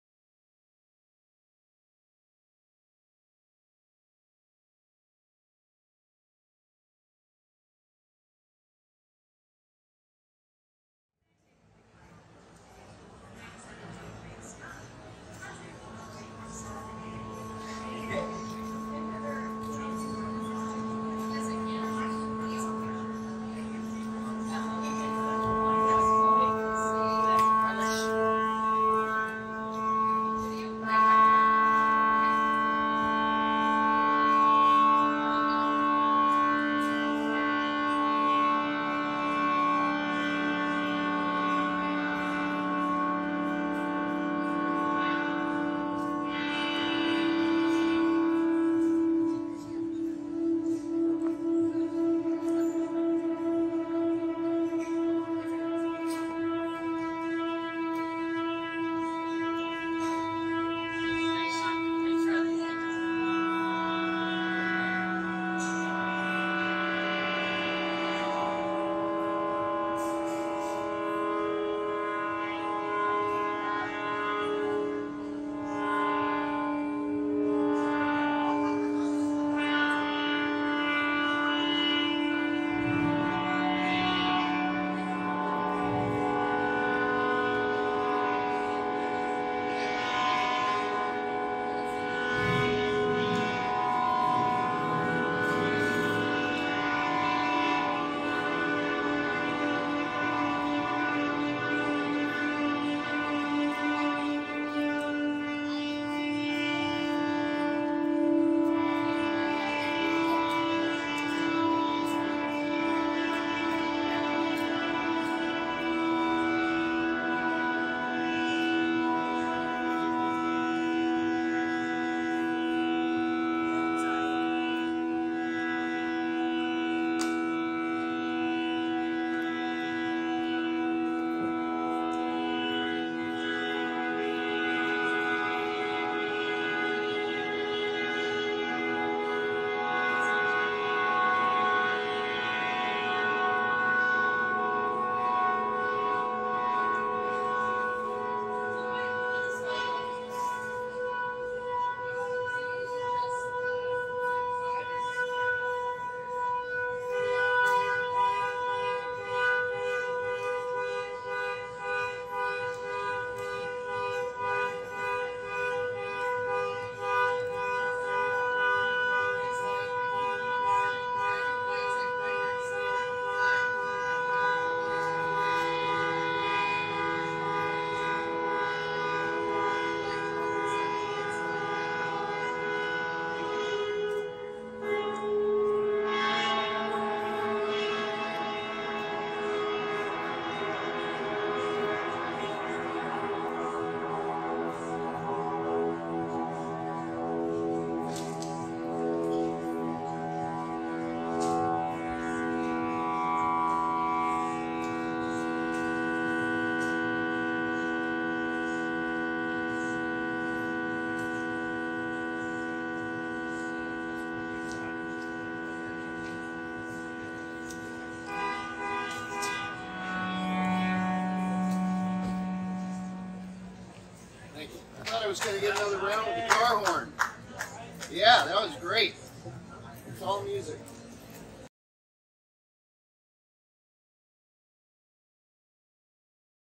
GNV STREET SHOWS - TOM MILLER PLAYS THE IPAD - CARHORNS
Video by Alexander Davidowski who happened to be passing by and agreed to shoot Tom Miller Playing the iPad on his iPhone.

Sept. 24th - Downtown Gainesville, Florida - the Known Center of the Universe.